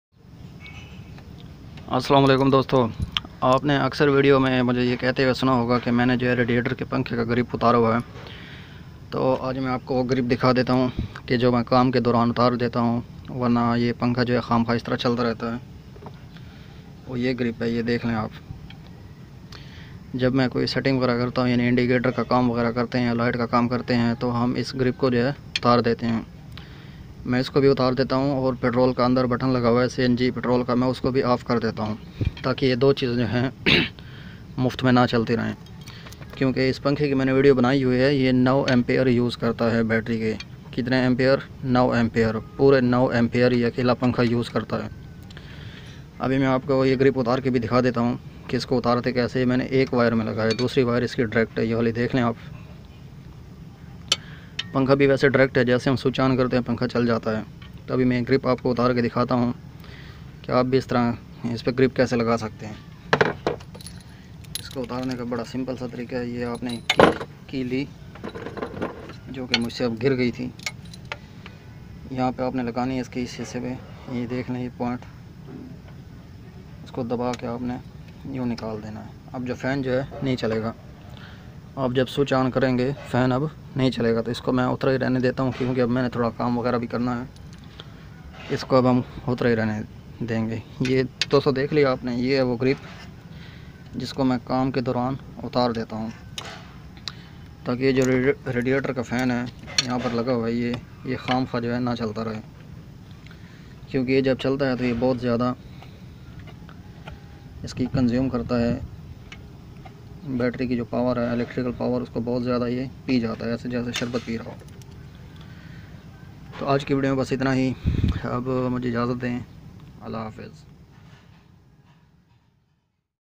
radiator fan grip for the daewoo racer car | how to make the radiator fan connection with the grips
this is a short video which is about the radiator fan grip daewoo racer car that how can place a connector or two points jack to fan supply line so when we need to run and during working in the can we be cut off the the supply connection easily